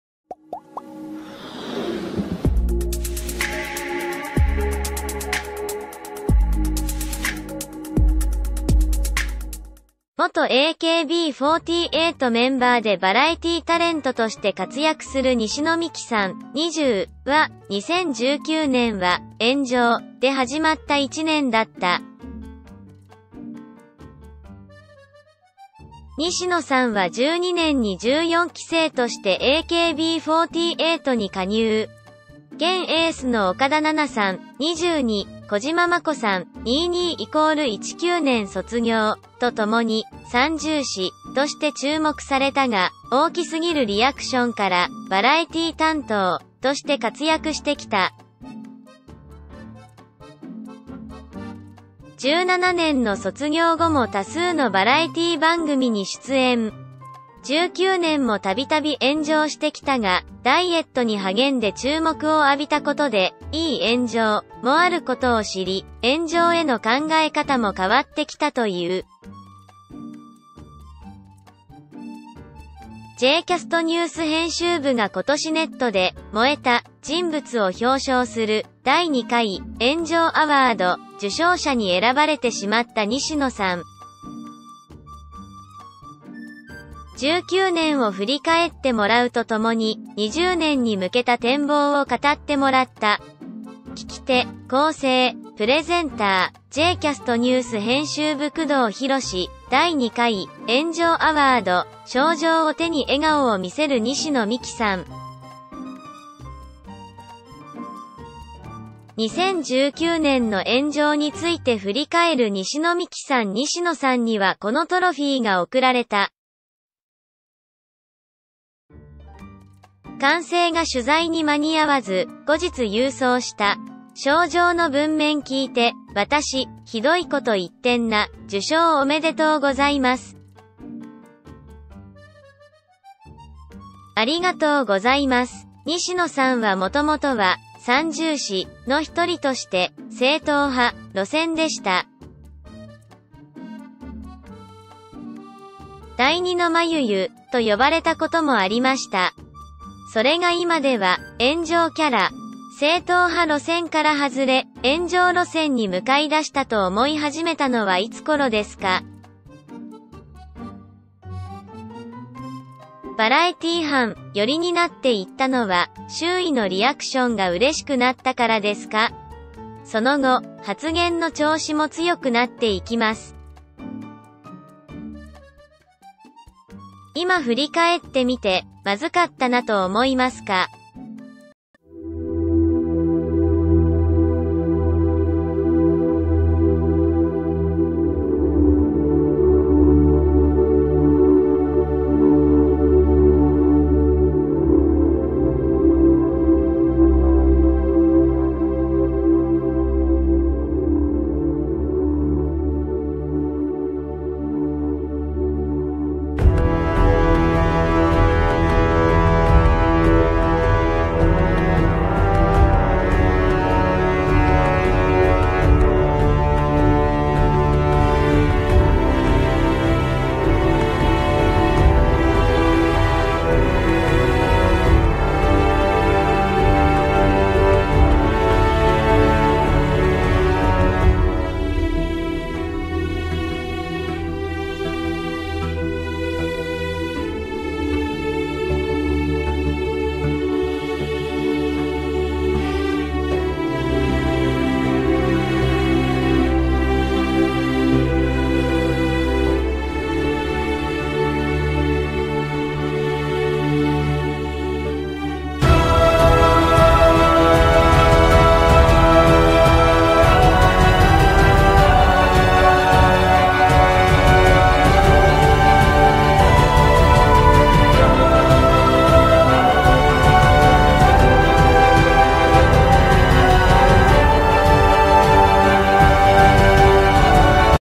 ✅ 元AKB48メンバーでバラエティータレントとして活躍する西野未姫さん（20）は、2019年は「炎上」で始まった1年だった。西野さんは12年に14期生としてAKB48に加入。現エースの岡田奈々さん
✍      元AKB48メンバーでバラエティータレントとして活躍する西野未姫さん（20）は、2019年は「炎上」で始まった1年だった。     西野さんは12年に14期生としてAKB48に加入。現エースの岡田奈々さん（22）、小嶋真子さん（22=19年卒業）とともに「三銃士」として注目されたが、大きすぎるリアクションから「バラエティー担当」として活躍してきた。17年の卒業後も多数のバラエティー番組に出演。19年もたびたび炎上してきたが、ダイエットに励んで注目を浴びたことで「いい炎上」もあることを知り、炎上への考え方も変わってきたという。     J-CASTニュース編集部が今年ネットで「燃えた」人物を表彰する、第2回「炎上アワード」受賞者に選ばれてしまった西野さん。19年を振り返ってもらうとともに、20年に向けた展望を語ってもらった。 （聞き手・構成・プレゼンター／J-CASTニュース編集部　工藤博司）  第2回「炎上アワード」賞状を手に笑顔を見せる西野未姫さん。  2019年の炎上についてふり返る西野未姫さん  西野さんにはこのトロフィーが贈られた。完成が取材に間に合わず、後日郵送した。   賞状の文面聞いて「私、ひどいこと言ってんな」   ――　受賞おめでとうございます。   ――　ありがとうございます。西野さんは元々は「三銃士」のひとりとして「正統派」路線でした。「第二のまゆゆ」と呼ばれたこともありました。それが今では「炎上キャラ」。正統派路線から外れ、炎上路線に向かいだしたと思い始めたのはいつ頃ですか。   ――　「バラエティー班」寄りになっていったのは、周囲のリアクションが嬉しくなったからですか？   ――　その後、発言の調子も強くなっていきます。   ――　今振り返ってみて、まずかったなと思いますか。   ...